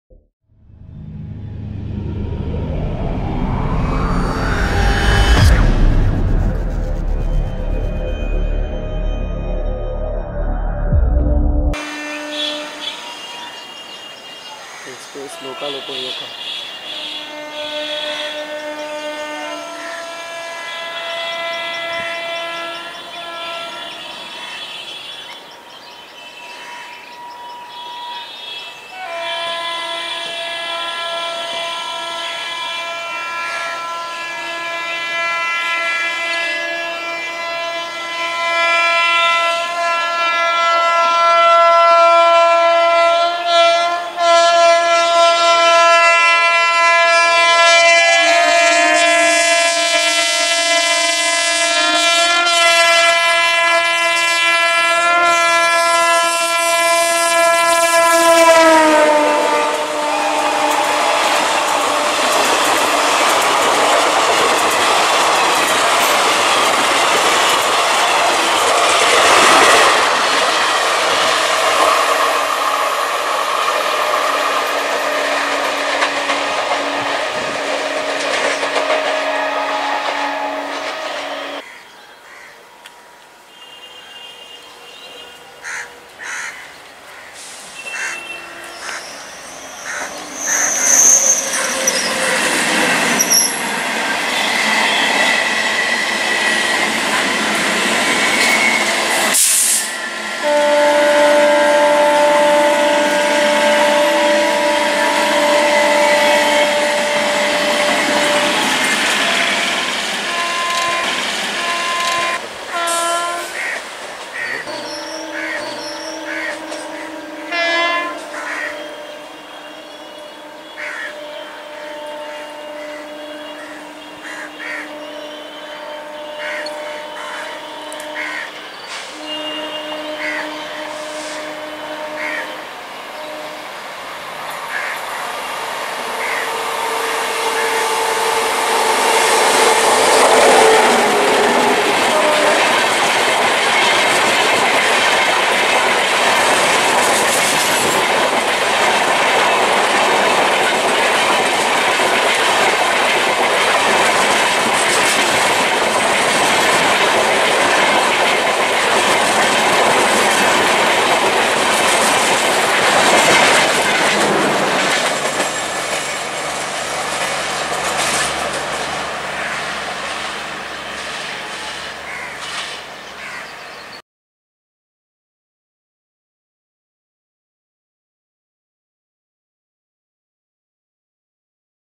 EMU Local Train made parallel run with Myurakshi FP but next EMU crossed with Viswabharati FP
Viswabharati Fast Passenger is one of the prominent trains of Indian Railways that runs between Rampurhat junction and Howrah junction. Total distance covered by the train is 44km/hr.
Mayurakshi Fast Passenger train runs between Howrah Junction and Rampur Hat Junction. This train departs ay 05:30 from the source station and arrive at the destination station at 11:35 on 7 days a week.